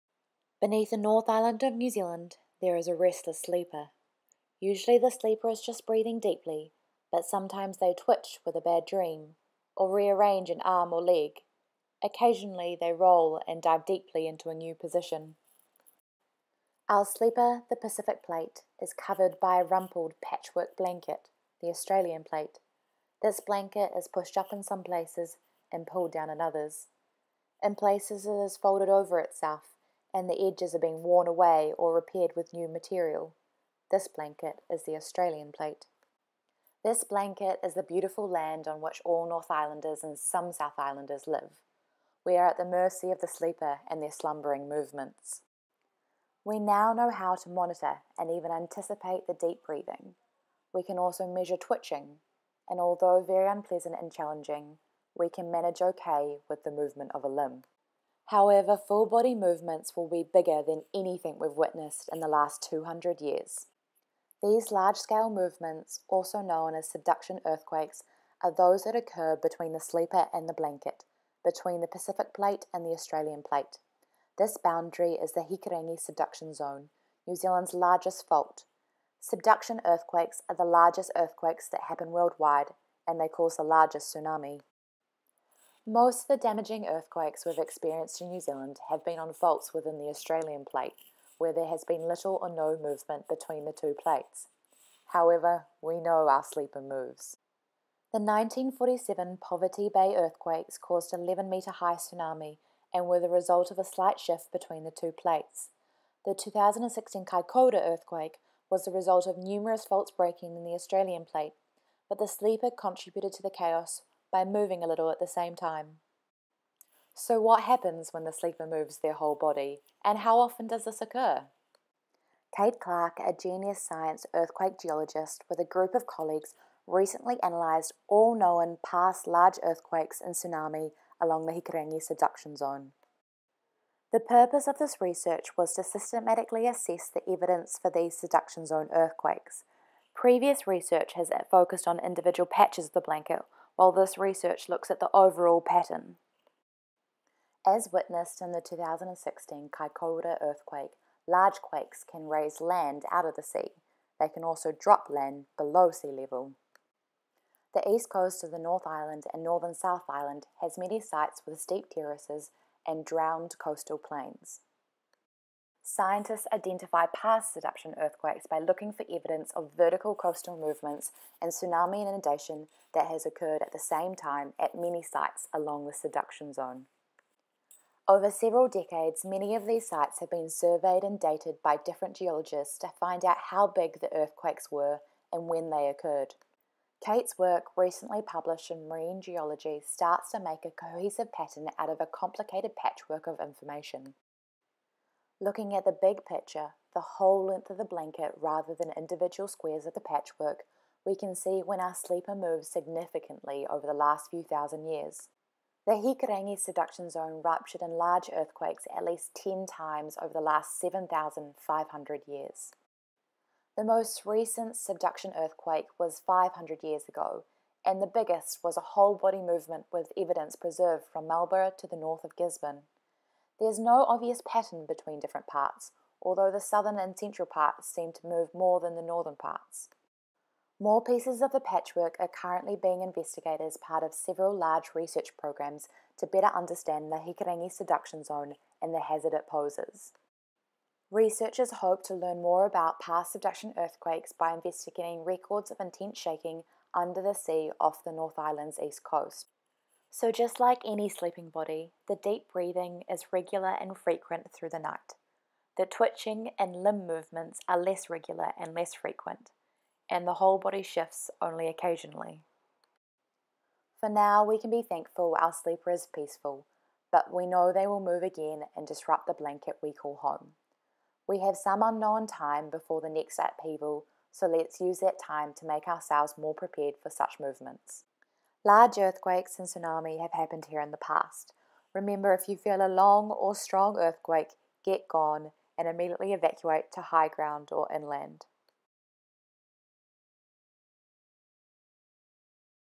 Hikurangi subduction zone: Stitching together the evidence
Beneath the North Island of New Zealand there is a restless sleeper. Our sleeper, the Pacific Plate, is covered by a rumpled patchwork blanket: the Australian Plate

Kate Clark with a group of her colleagues recently pieced together all known past large earthquakes and tsunami along the Hikurangi subduction zone to figure out when and where they occurred.

Watch to learn more about our restless sleeper's history of movements along the North Island's east coast.